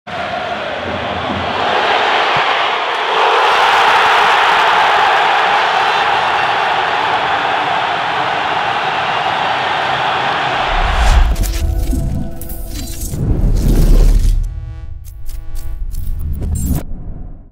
Racing-LOSC (1-2) : le but d'Ibrahima Sissoko vu du bord terrain
Entré en cours de jeu, Ibrahima Sissoko a inscrit son 4e but sous le maillot du Racing. Une réalisation qui a permis au Racing de croire jusqu'au bout à l'égalisation.